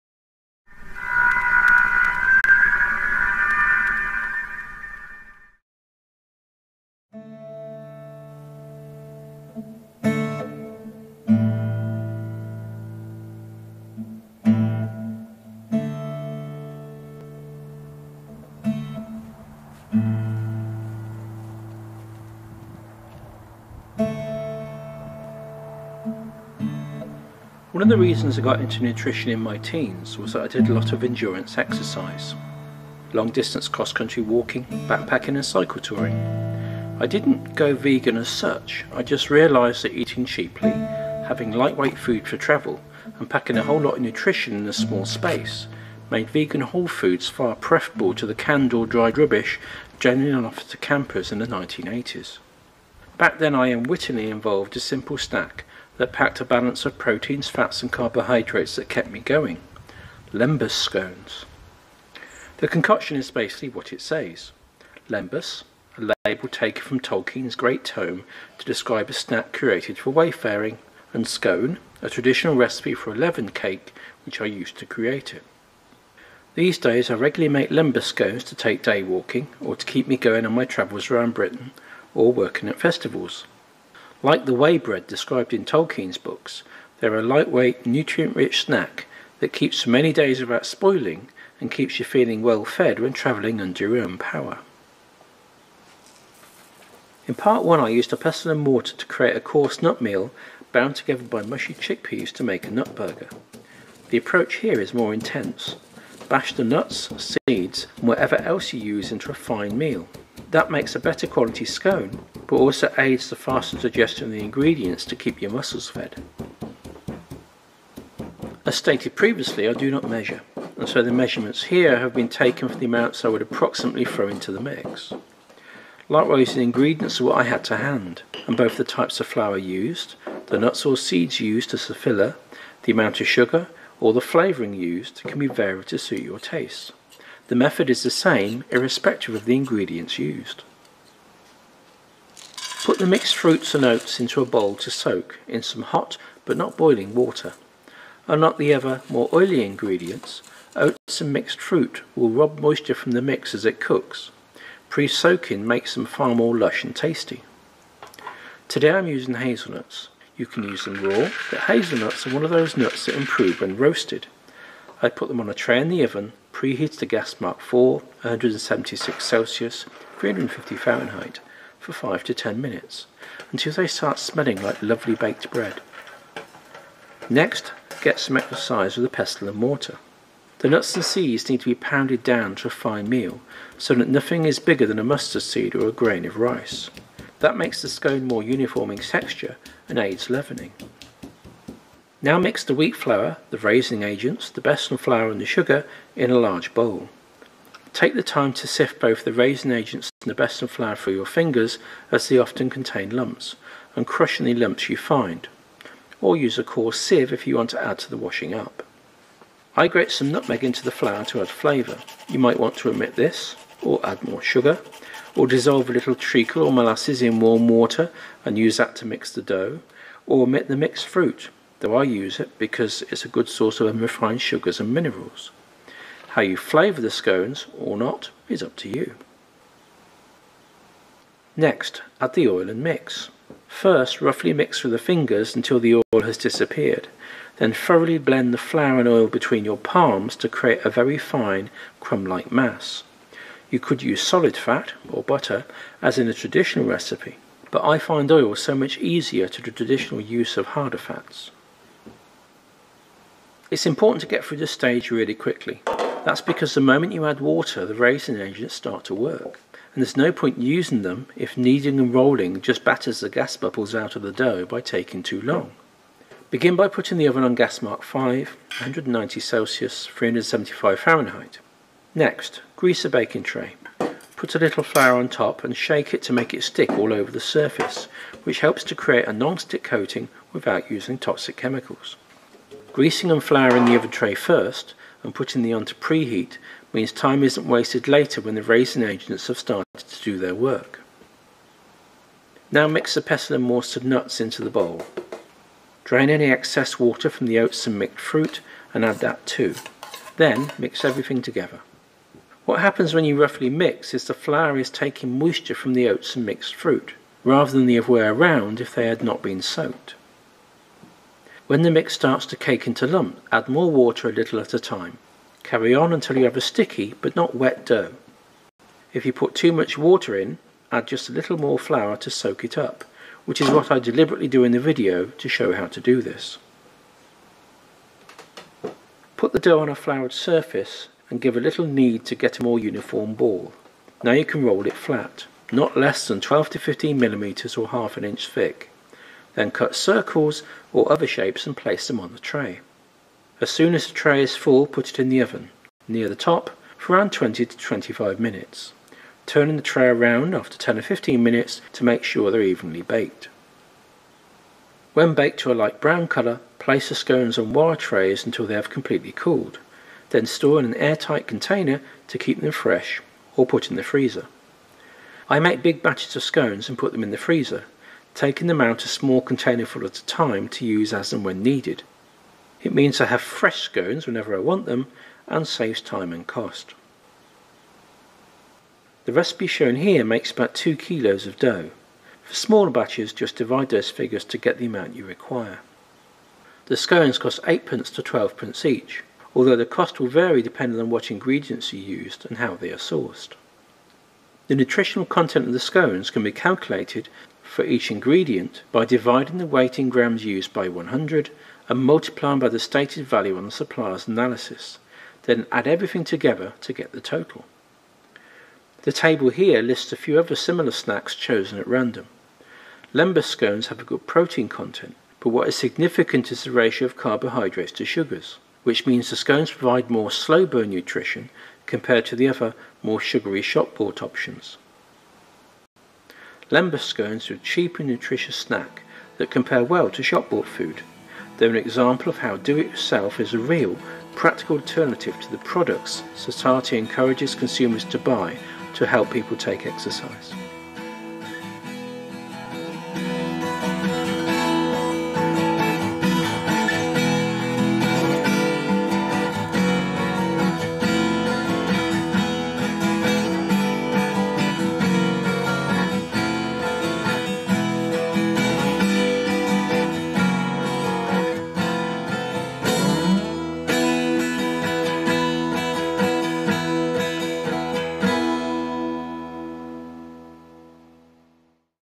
Part 2: ‘Lembas Scones, my ideal (vegan) wayfaring snack’ – ‘An Anarchist’s Cookbook’
(Ramblinactivist 2021/6)

In this video I show how to make a cheap and simple snack food that’s ideal for keeping well fed when travelling or working.

One of the reasons I got really into nutrition in my teens was that I did a lot of endurance exercise – long distance cross-country walking, backpacking, and cycle touring. I didn’t “go vegan” as such; I just realised that eating really cheaply, having light-weight food for travel, and packing a whole lot of nutrition in a small space, made vegan wholefoods far preferable to the canned or dried rubbish generally on offer to campers in the 1980s.

Back then I (unwittingly) evolved a simple snack that packed a balance of protein, fats, and carbohydrates that kept me going: “Lembas Scones”.

I show how to make them, and I do an analysis to show how they compare to the common ‘shop-bought’ energy/protein snacks on the market.

As I show, “Do-It-Yourself” is a far better option to help you live a simple, cheap and low impact lifestyle.

You can find the full ingredients list and instructions in the ‘An Anarchist’s Cookbook’ blog at: